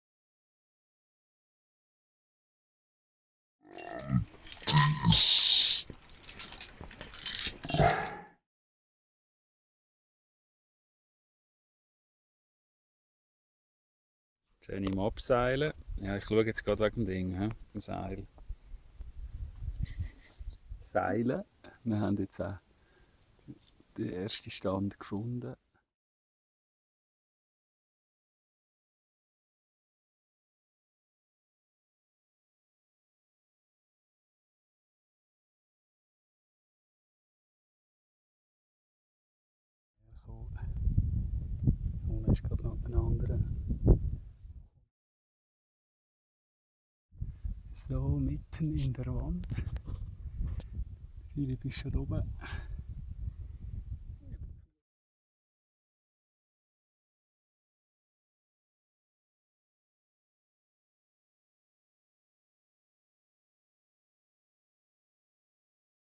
Climbing in the Alpstein - Kluckerplatte
Climbing in the Alpstein - Kluckerplatte - Route: Eine halbe Portion Appenzeller